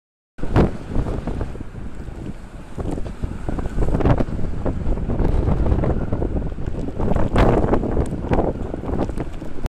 Blæsten tvinger Stena Saga til at skifte side i færgeleje
Da jeg var ude på havnen var der rigtigt dårligt vejr.Middel vindhastighed på 25 m/s efter havnens vejrstation. Godt der var l ved den gamle Larvik terminal.